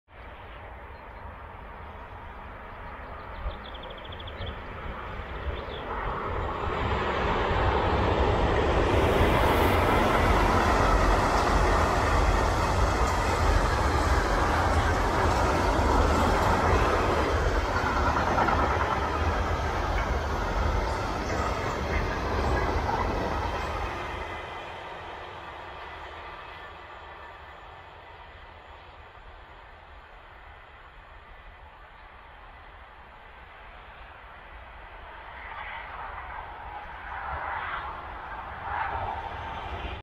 [4K] Siemens Vectron Dual Mode 248 004 am DGS 52560
Schon ein paar Mal fuhr in 2021 die Vectron Dual Mode der Baureihe 248 Probe für RheinCargo - so auch am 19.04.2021.
248 004 war an diesem Tag mit dem DGS 52560 München-Milbertshofen - Ingolstadt Hbf bei Ossenzhausen in der Hallertau unterwegs.

248 004, hired to RheinCargo, passes Ossenzhausen with a freight train direction Ingolstadt on 19 April 2021.

© ICE 91 Prinz Eugen 2021